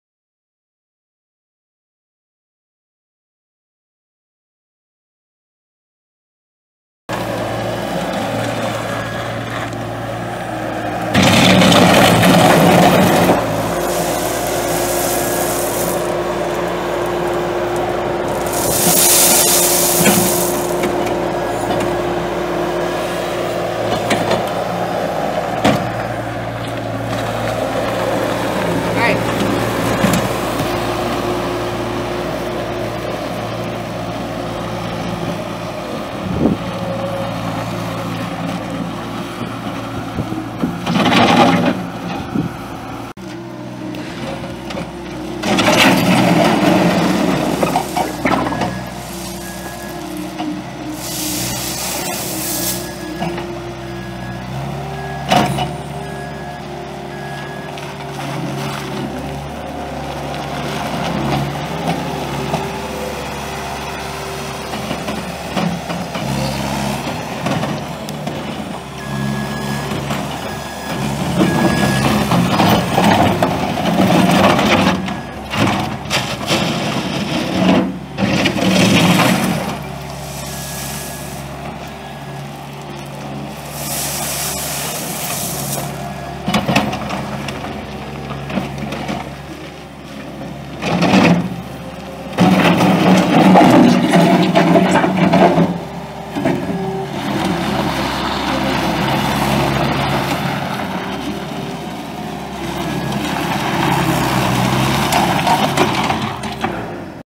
2011 Bobcat S185 skid steer for sale at auction | bidding closes June 14, 2018
2011 Bobcat S185 skid steer
1,337 hours on meter
Kubota V2607-D1-T-ET04 2.6L four cylinder turbo diesel engine
Two speed hydrostatic transmission
Enclosed cab
EMS panel
Spring suspension seat
Block heater
68"W low profile bucket
1,850 lbs rated operating capacity
30 x 10-16 solid tires
Door lift support will be installed prior to auction
Rear lights missing
Serial A3L940489
It's located in Superior, NE, and we're selling it to the highest bidder regardless of price.